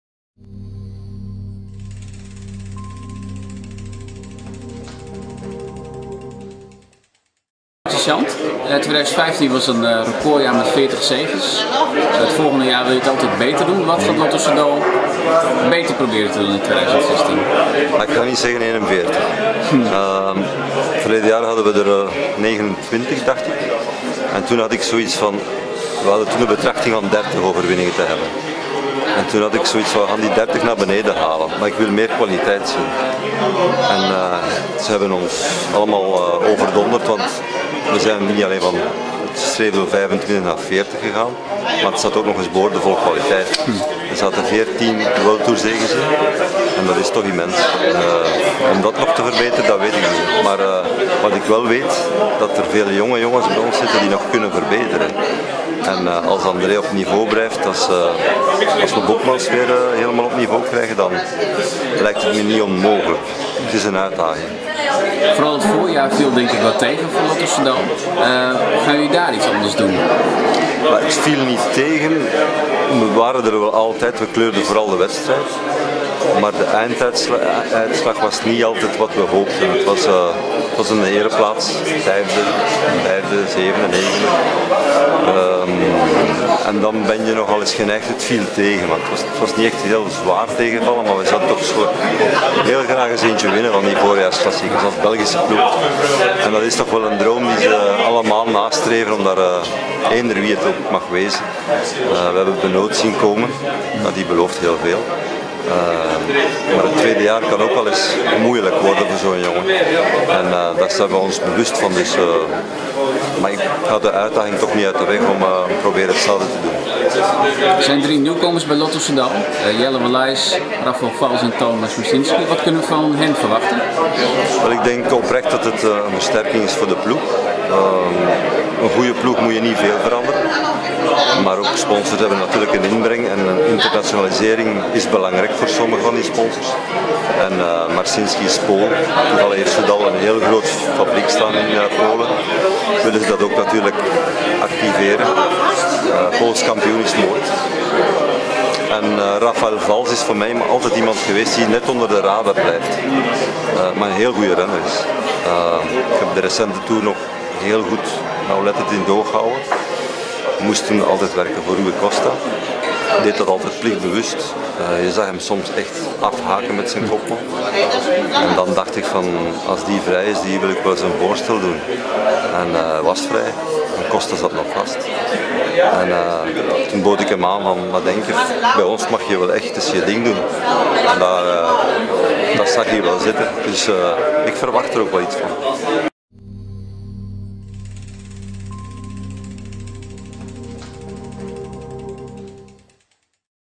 Marc Sergeant: 'Volgend jaar Belgische voorjaarsklassieker winnen'
Wieler Revue woont het trainingskamp van Lotto Soudal op Mallorca bij en blikte met manager Marc Sergeant terug en vooruit. Lotto Soudal heeft het afgelopen wielerseizoen een recordjaar achter de rug met maar liefst 40 overwinningen.